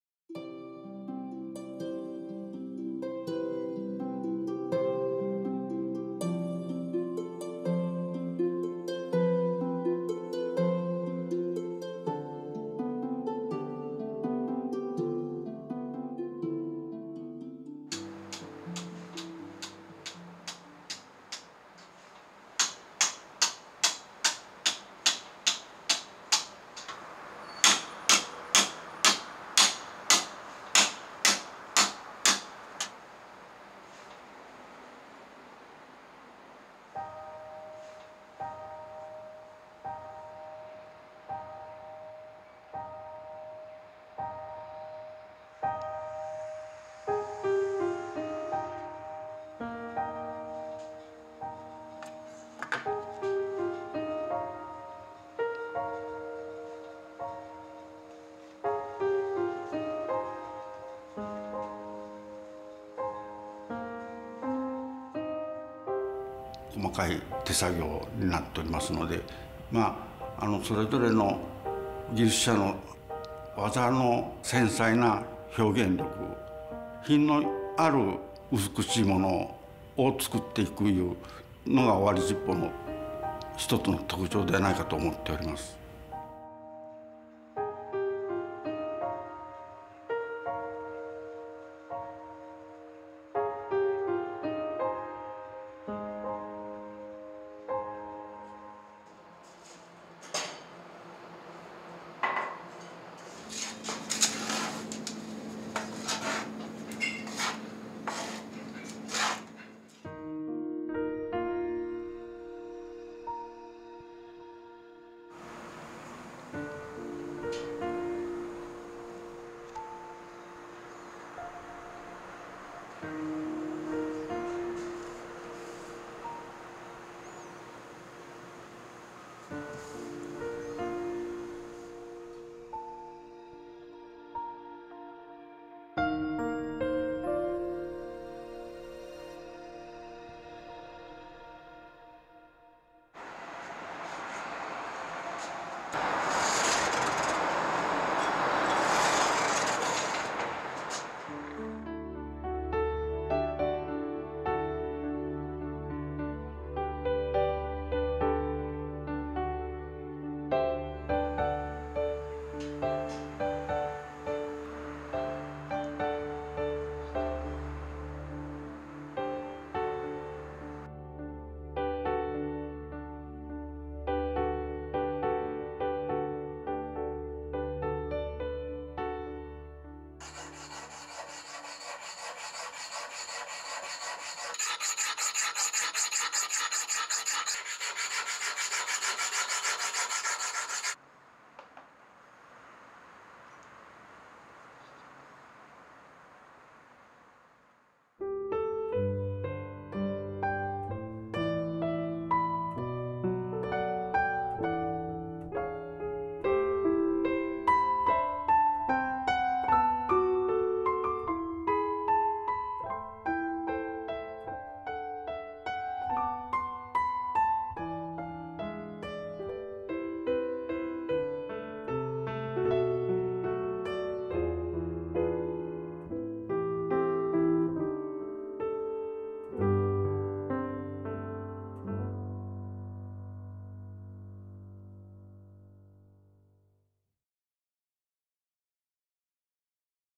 手技TEWAZA「尾張七宝」Owari Cloisonne／伝統工芸 青山スクエア Japan traditional crafts Aoyama Square
尾張七宝の起こりは、江戸時代後期に、現在の名古屋市を中心とした地域にあたる尾張の梶常吉（かじつねきち）が作ったのが始まりとされ、常吉によって技術・技法が確立されました。

The origins of Owari cloisonné date back to the Edo era (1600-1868) when the first pieces were made in the area ruled by the Owari clan, in what is now Nagoya.